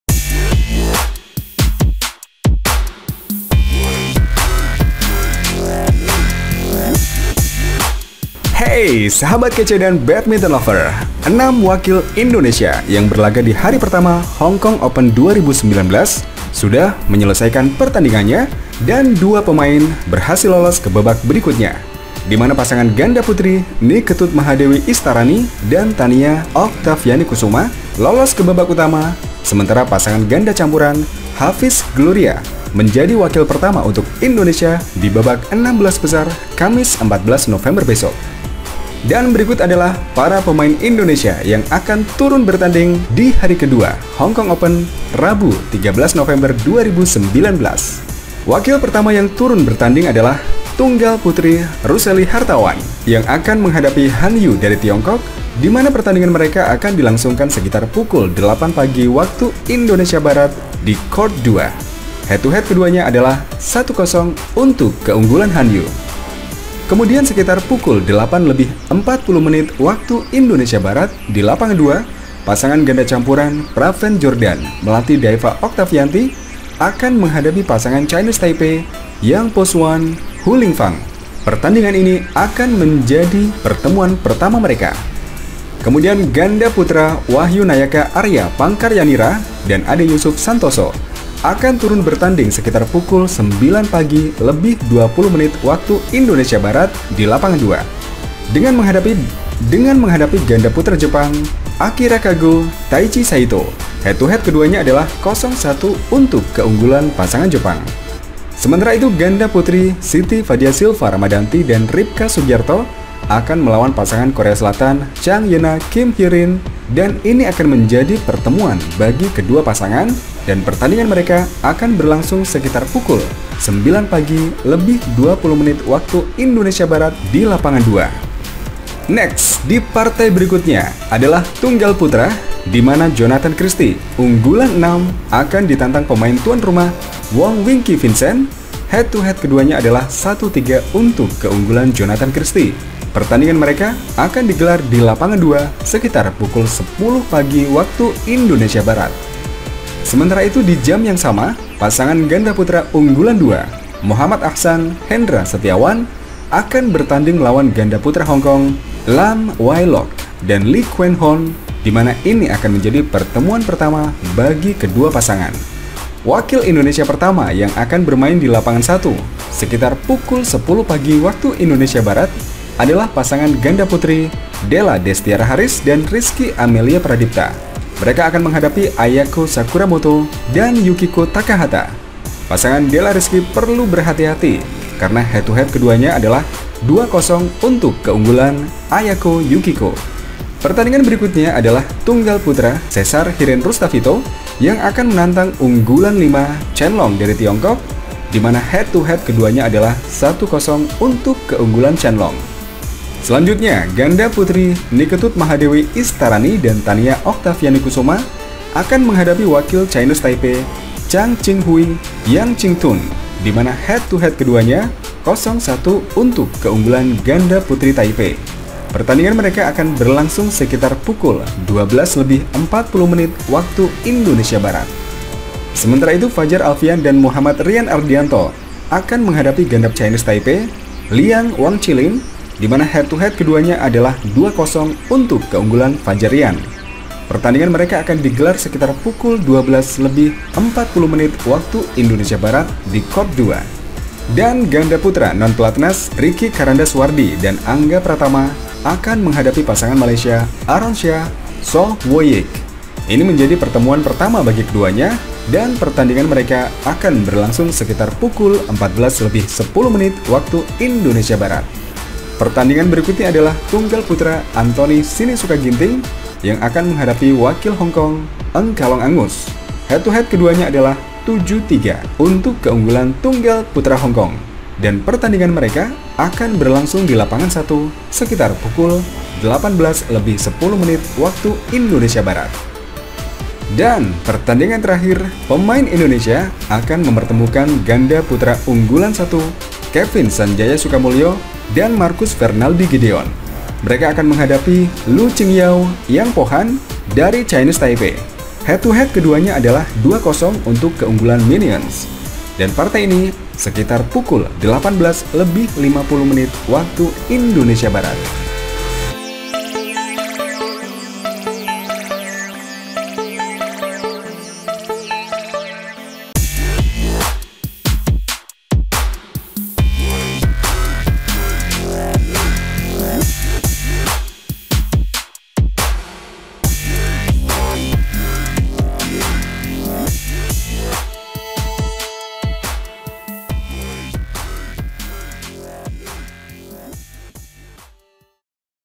JADWAL BABAK 32 BESAR RABU 13 NOV - HONGKONG OPEN 2019
Sahabat kece.. 6 wakil Indonesia yang berlaga dihari pertama Hongkong Open 2019 sudah menyelesaikan pertandingannya  dan 2 wakil berhasil lolos kebabak berikutnya..dimana pasangan ganda putri Ni Ketut Mahadewwi Istarai/Tania Oktaviani Kusumah lolos kebabak utama , sementara itu pasangan Ganda campuran Hafiz/Gloria menjadi wakil pertama untuk indonesia dibabak 16 Besar kamis 14 nov Besok..
Dan berikut adalah para pemain Indonesia yang akan turun bertanding dihari kedua Hongkong Open, Rabu 13 November 2019
Wakil pertama yang turun bertanding adalah Tunggal Putri Ruselli Hartawan yg akan menghadapa Han Yue dari tiongkok sekitar pukul 8 pagi WIB di Court 2 , H2H keduanya adalah 0 : 1 untuk keunggulan Han Yue
Kemudian sekitar pukul 08.40 WIB dilapangan 2 , pasangan ganda campuran Praveen Jordan/melati daeva Oktavianti akan menghadapi pasangan Chinese Taipei Yang Po-Hsuan/HU Ling fang , ini menjadi pertemuan pertama untuk kedua pasangan ini
Kemudian ganda putra Wahyu Nayaka Arya Pangkaryanira/Ade Yusuf santoso akan turun bertanding sekitar pukul 09.20 WIB dilapangan 2 dengan menghadapi ganda putra jepang Akira Kago/Taichi SAITO , H2H keduanya 0 : 1 untuk keunggulan Pasangan Jepang
Sementara itu ganda putri Siti fadia Silva ramadhanti /Ribka Sugiarto akan melawan Pasangan Korea selatan CHANg YE NA/KIM Hye Rin, ini juga menjadi pertemuan pertama bagi kedua pasangan, pertandingan mereka akan berlangsung sekitar pukul 09.20 WIB di court 2
Next ..dipartai berikutnya adalah tunggal putra dimana Jonatan Christie Unggulan 6 akan ditantang pemain Tuan Rumah Wong Wing ki Vincent , H2H keduanya 1 :3 untuk keunggulan Jonatan Christie..pertandingan akan digelar di Court 2 sekitar jam 10 Pagi WIB
Sementara itu di jam yang sama pasangan ganda putra Unggulan 2 Mohammad Ahsan/Hendra Setiawan akan bertanding melawani Ganda Putra Hongkong  LAM Wai Lok/LI Kuen HOn.. ini menjadi pertemuan pertama bagi kedua pasangan
Wakil Indonesia pertama yang bermain dilapangan 1 sekitar jam 10 Pagi adalah pasangan ganda Putri Della Destiara haris/Rizki Amelia Pradipta vs Ayako Sakuramoto/Yukiko Takahata , H2H keduanya adalah 2 : 0 untuk keunggulan pasangan Ayako/Yukiko
Pertndingan b erikutnya adalah Tunggal putra Shesar hiren Rhustavito yang akan menantang unggulan 5 chen long dari Tiongkok. H2H keduanya adala 1 :0 untuk keunggulan Chen Long
Seloanjutnya Ganda Putri Ni Ketut Mahadewi Istarani/Tania Oktaviani Kusumah akan menghadapi wakil Chinese Taipei  CHANG Ching Hui/YANg Ching Tun, dimana H2h keduanya 0:1 untuk keunggulAN GANDA PUTRI TAIPEI..pertandingan mereka akan berlangsung sekitar pukul 12.40 WIB
Sementara itu fajar Alfian/Muhammad Rian Ardianto akan menghadapi ganda putra Chinese Taipei LEE Yang /Wang Chi Lin, h2h keduanya 2 : 0 untuk keunggulaN  FAJAR/Rian, Pertandingan mereka kan digelar sekitar pukul 12.40 WIB di court 2
Dan ganda putra Non Pelatnas Ricky Karandasuwardi/Angga Pratama akan menghadapi pasangan Malaysia Aaron Chia/SOH Wooi Yik .dan ini menjadi pertemuan  pertama bagi keduanya , pertandinganb mereka akan berlangsung sekitar pukul 14.10 WIB
Pertandingan berikutnya dalah tunggal putra Anthony sinisuka Ginting vs NG Ka Long Angus , H2h keduanya adalah 7 ;3 untuk keunggulan pemain hongkong tersebut dan pertandingan mereka kana berlangsung di lapangan 1 sekitar pukul 18.10 WIB
Dan pertandingan terakhir akan mempertemukan ganda putra unggulan 1 Kevin sanjaya sukamuljo/Marcus fernaldi Gideon vs LU Ching Yao/YANg Po Han , H2H keduanya 2 :0 untuk keunggulan Minions.. partai ini akan berlangsung di lapangan 1sekitar jam 18.50 WIB..